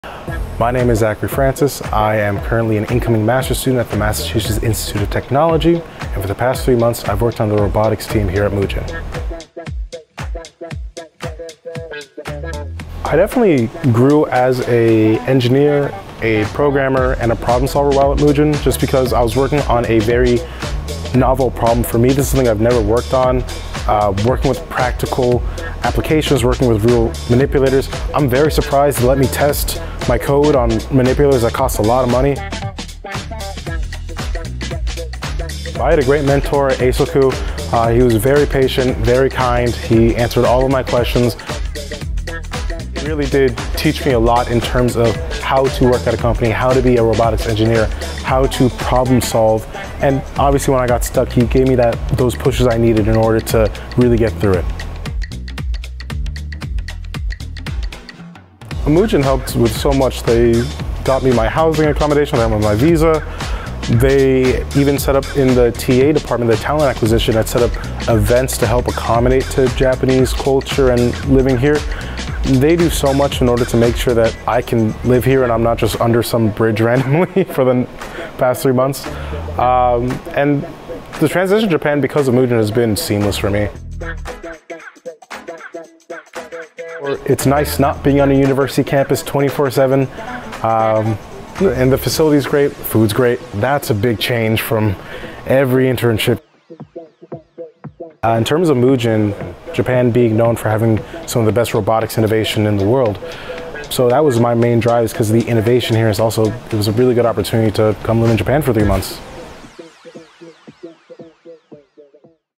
Zachary Francis, Robotics Intern at Mujin
From MIT to Mujin: A Robotics Intern’s Perspective